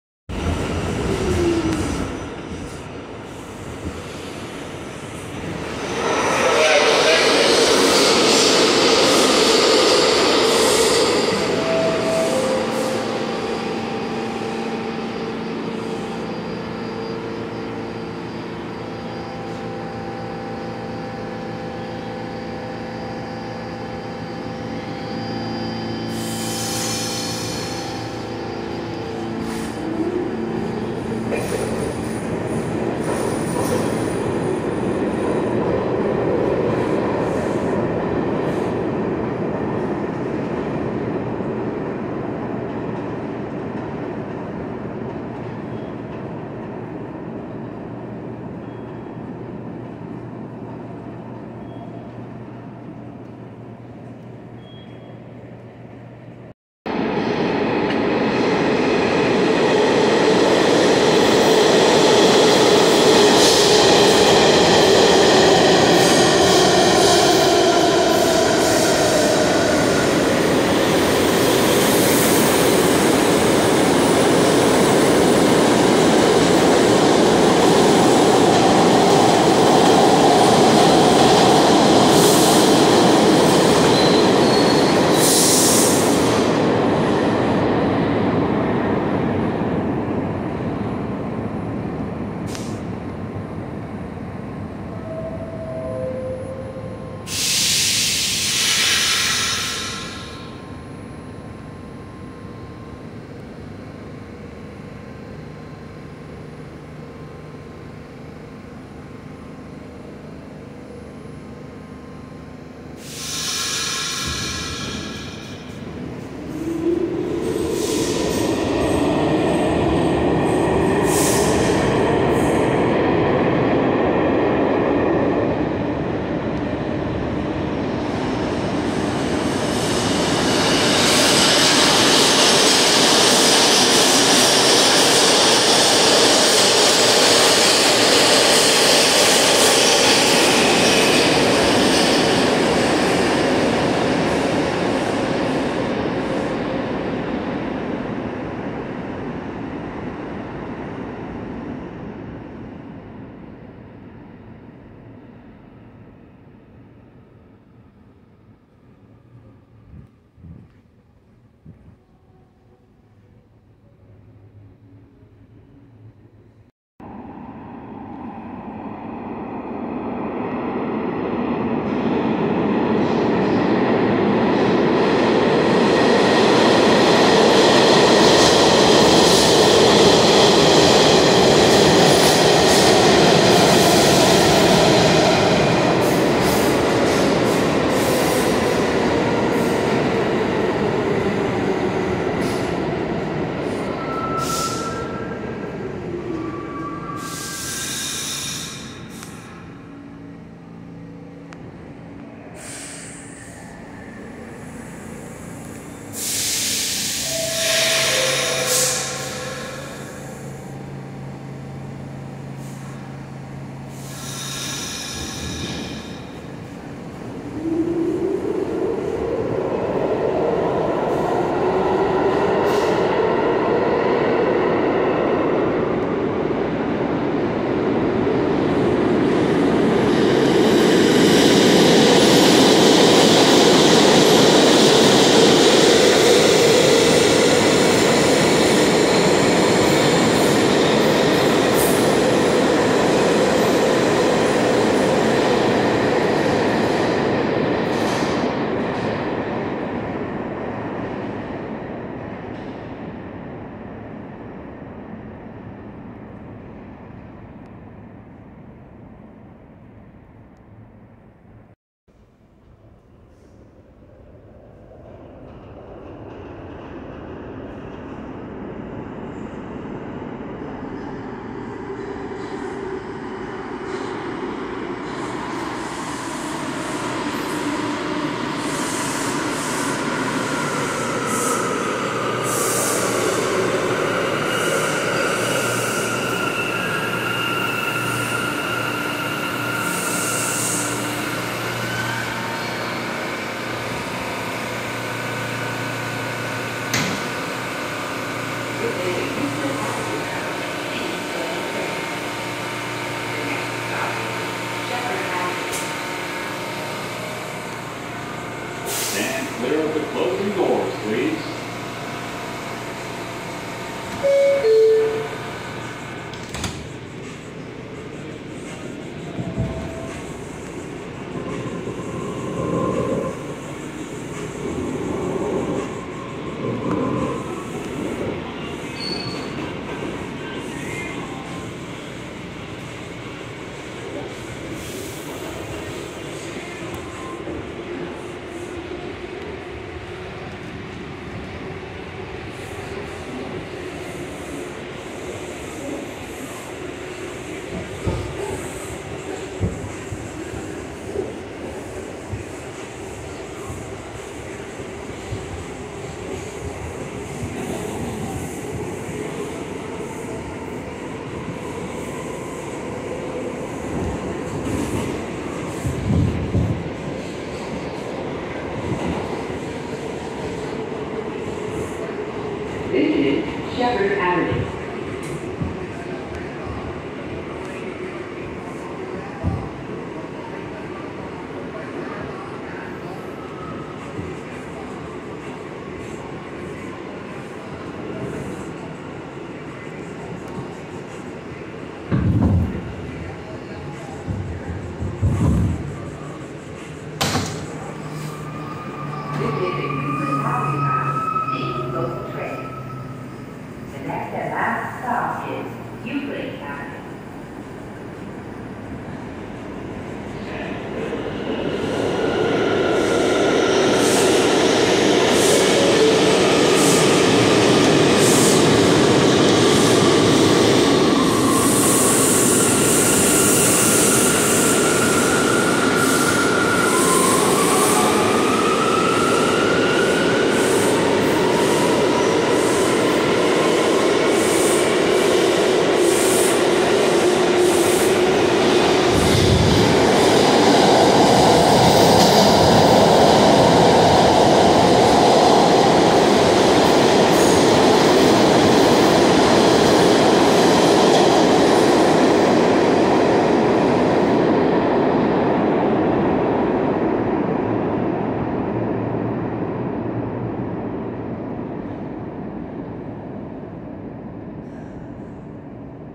MTA NYC Subway|IND Fulton Street Line|R46 & R179's (A) Exp & (C) Lcl Trains at Van Siclen Avenue
A & C Trains at Van Siclen Avenue

This video was Taken 6/13/2022 at 9:04 P.M.

This Station Opened 11/28/1948